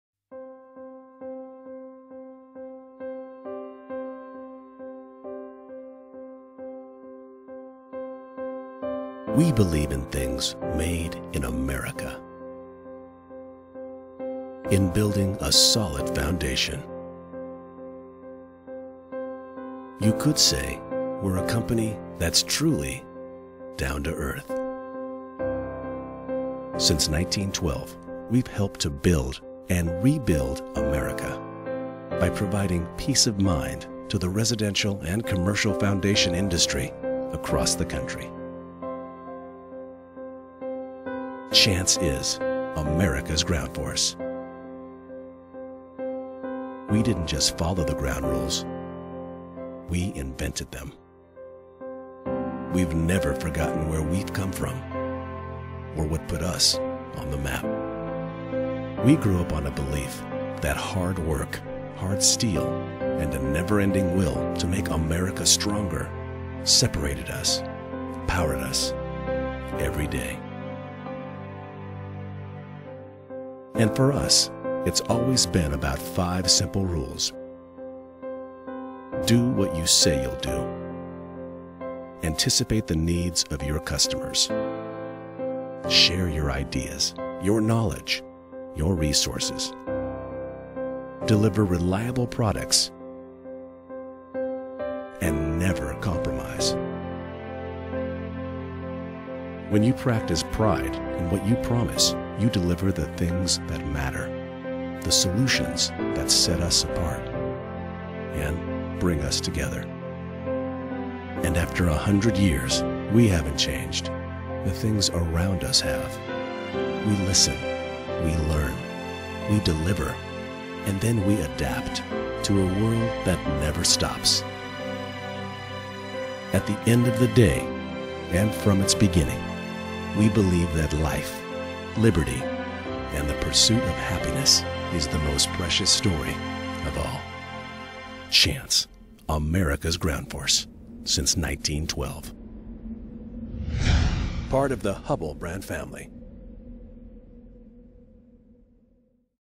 A.B. Chance - Helical Piles.wmv
AB Chance manufactures the industry leading helical piers. EBS Geostructural installs Chance Helical Piers in Ontario.